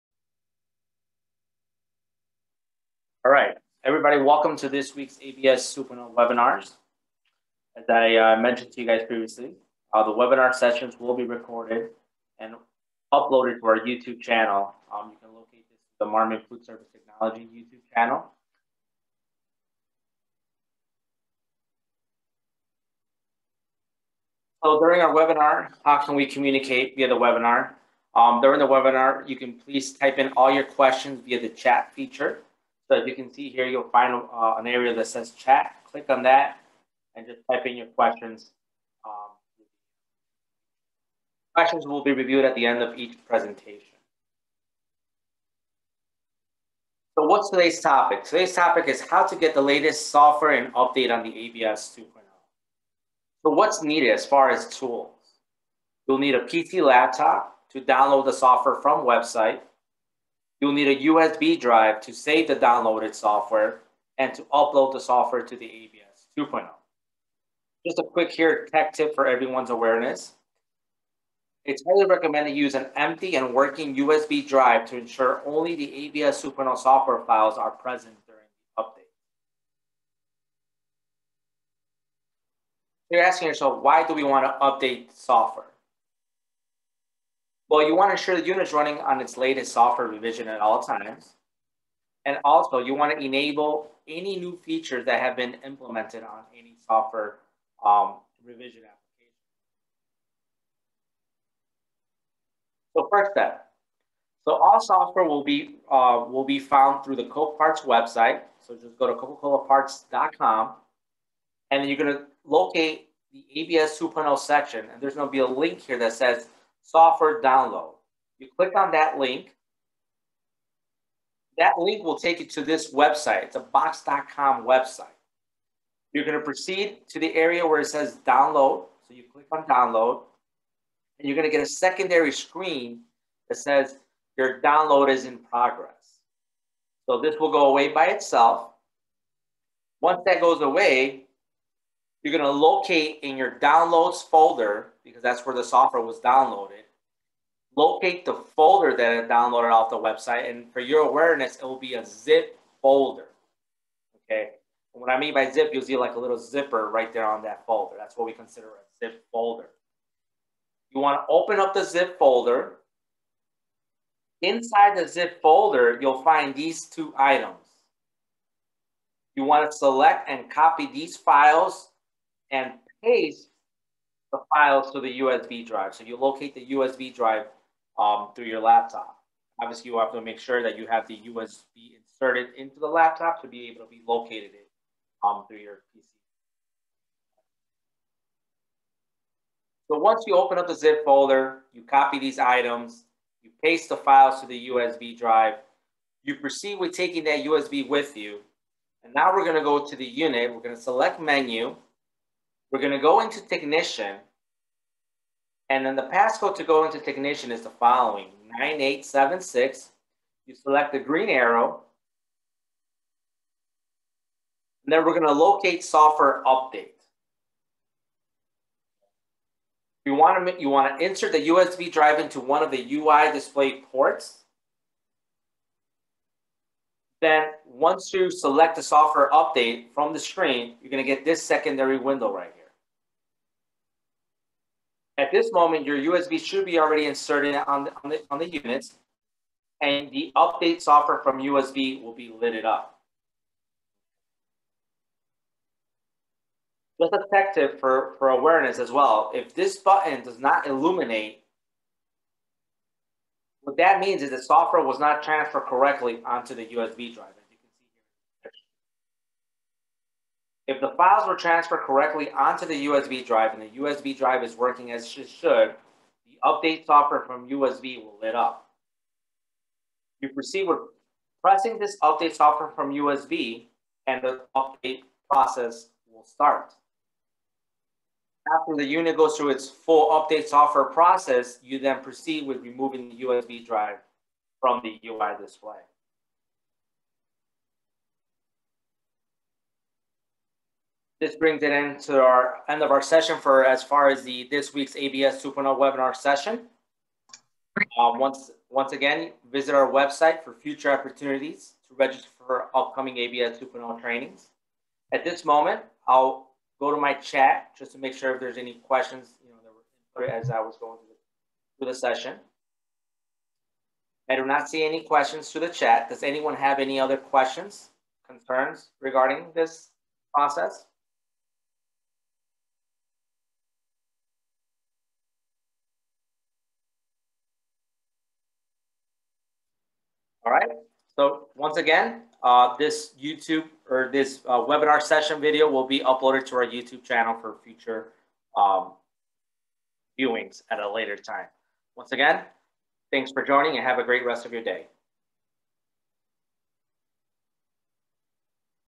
ABS 2.0 Software Download and Upload
How to Download and Upload ABS 2.0 Software.. Start to Finish...